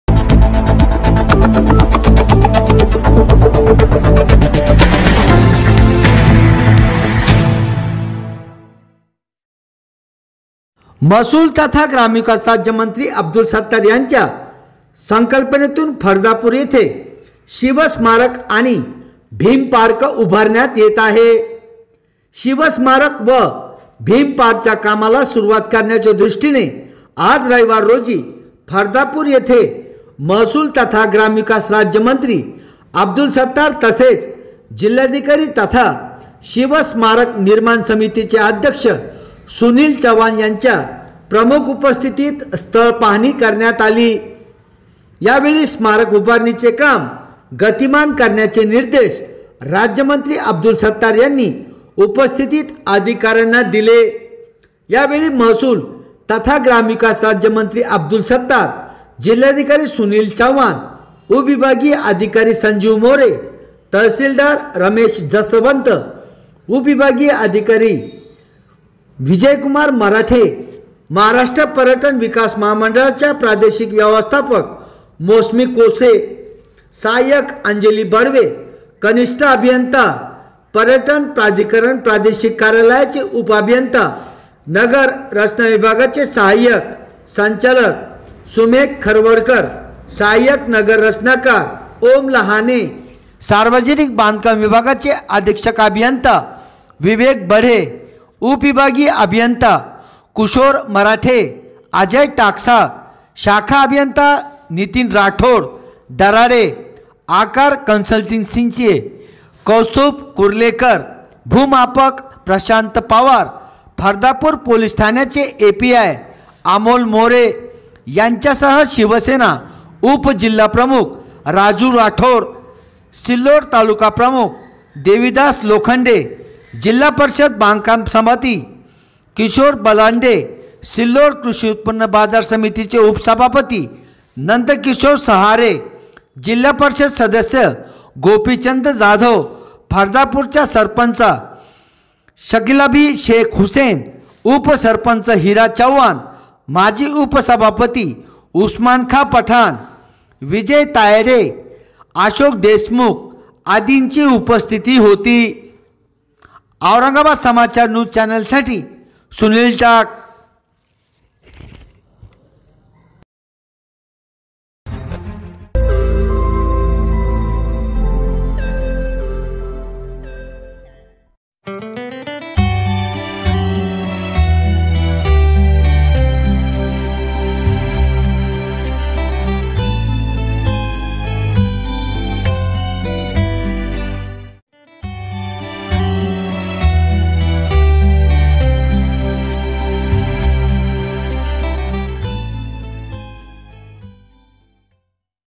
राज्यमंत्री अब्दुल सत्तार आणि जिल्हाधिकारी सुनील चव्हाण यांनी केली शिवस्मारक-भीम पार्क कामाची पाहणी
ब्रेकिंग !! राज्यमंत्री अब्दुल सत्तार आणि जिल्हाधिकारी सुनील चव्हाण यांनी केली शिवस्मारक-भीम पार्क कामाची पाहणी !!